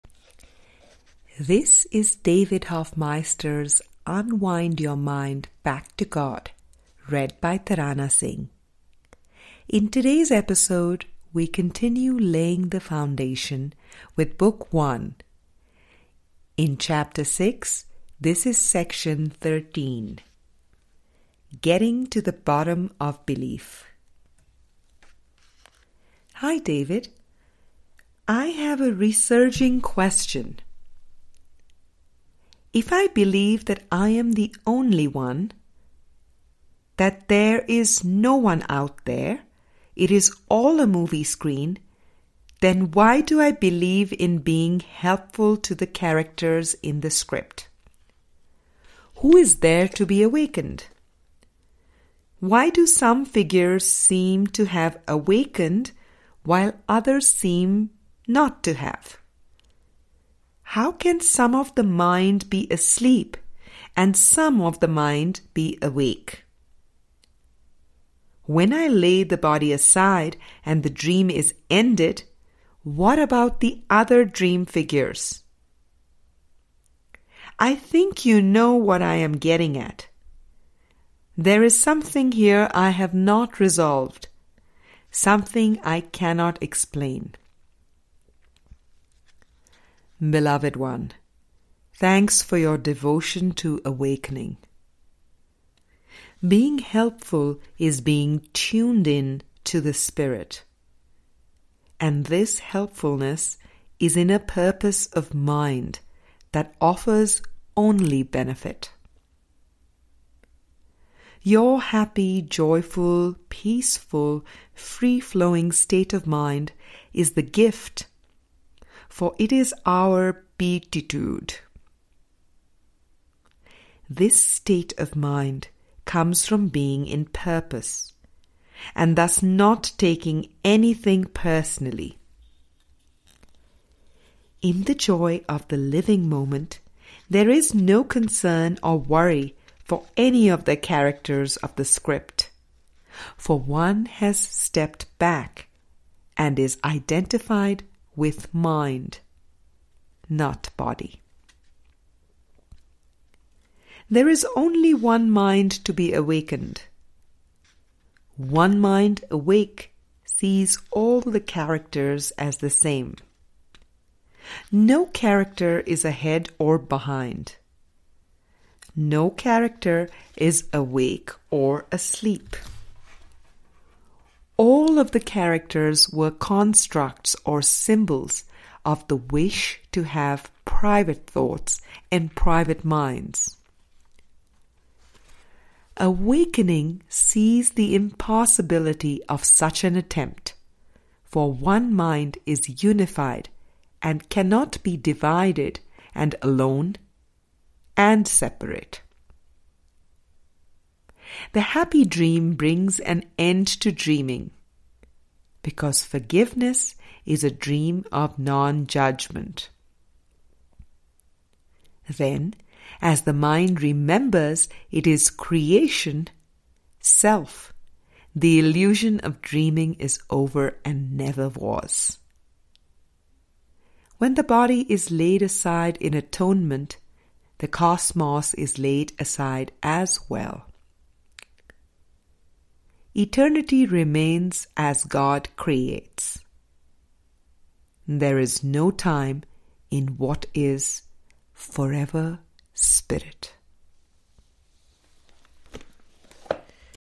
Unwind Your Mind Ch. 6 Sec. 13 - Getting to the Bottom of Belief - David Hoffmeister ACIM
This reading includes the thirteenth section of Unwind Your Mind Chapter. 6 ( Getting to the Bottom of Belief )  from Book One: Laying the Foundation.
This is the sixty third of several upcoming episodes featuring readings of this profound work of consolidated conversations between ACIM teacher David Hoffmeister and his students.